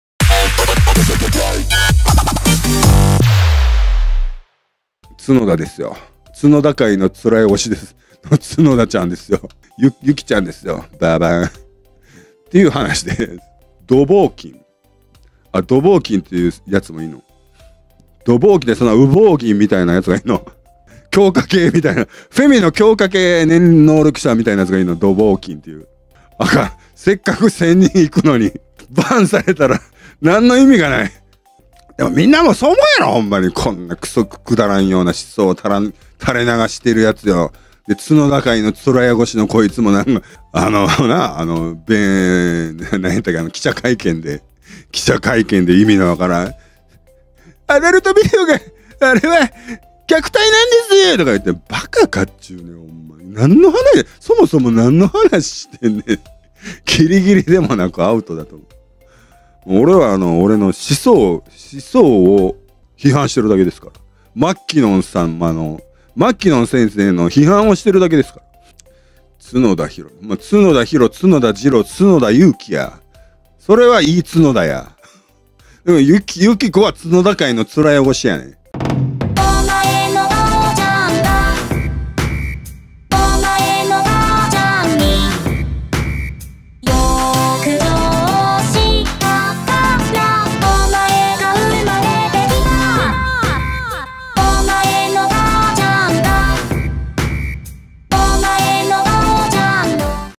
【仁藤夢乃Colabo】角田由紀子 弁護士「アダルト部瀬尾は女性への虐待なんですぅ！」←アホか！！(笑)
角田界の面汚し(笑)

【関連動画】
「Colaboと仁藤夢乃さんを支える会」のメンバーを見ていこうかと想像します